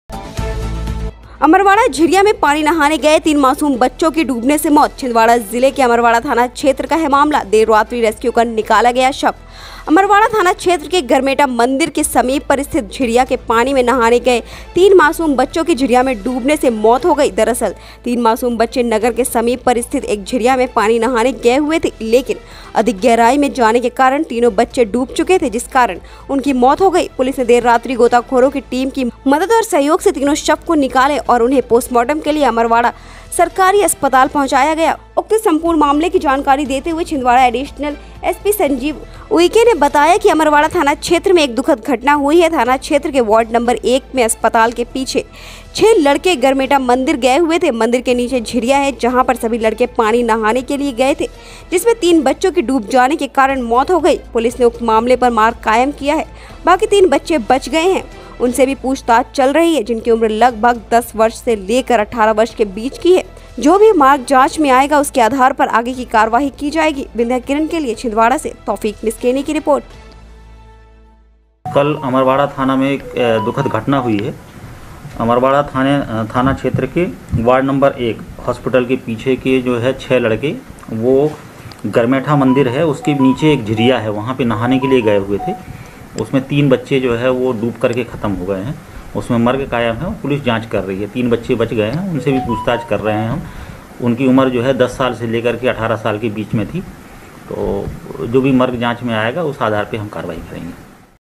छिदवाडा: पानी में नहाने गए तीन मासूम बच्चो की डूबने से मौत#VINDHYAKIRAN
अमरवाड़ा झिरिया में पानी नहाने गए तीन मासूम बच्चे की डूबने से मौत...छिंदवाड़ा जिले के अमरवाड़ा थाना क्षेत्र का है मामला...देर रात्रि रेस्क्यू कर निकाला गया शव

अमरवाड़ा थाना क्षेत्र के गरमेटा मंदिर के समीप पर स्थित झिरिया के  पानी में नहाने गए तीन मासूम बच्चो की झिरिया में डूबने से मौत हो गई।दरअसल तीन मासूम बच्चे नगर के समीप पर स्थित एक झिरिया में पानी नहाने गए हुए थे लेकिन अधिक गहराई में जाने के कारण तीनों बच्चे डूब चुके थे।जिस कारण उनकी मौत हो गई।पुलिस ने देर रात्रि गोताखोरो की टीम की मदद ओर सहयोग से तीनों शवो को निकाले और उन्हें पोस्टमार्टम के लिए अमरवाड़ा सरकारी अस्पताल पहुंचाया गया।उक्त संपूर्ण मामले की जानकारी देते हुए छिंदवाड़ा एडिशनल एस पी संजीव उईके ने बताया कि अमरवाड़ा थाना क्षेत्र में एक दुखद घटना हुई है थाना क्षेत्र के वार्ड नंबर एक में अस्पताल के पीछे के 6 लड़के गरमेटा मंदिर गए हुए थे मंदिर के नीचे झिरिया है जहा पर सभी लड़के पानी नहाने के लिए गए थे।जिसमे तीन बच्चो की डूब जाने के कारण मौत हो गई पुलिस ने उक्त मामले पर मर्ग कायम किया है बाकी तीन बच्चे बच गए है उनसे भी पूछताछ चल रही है जिनकी उम्र लगभग 10 वर्ष से लेकर 18 वर्ष के बीच की है जो भी मर्ग जांच में आएगा उसके आधार पर आगे की कार्यवाही की जाएगी।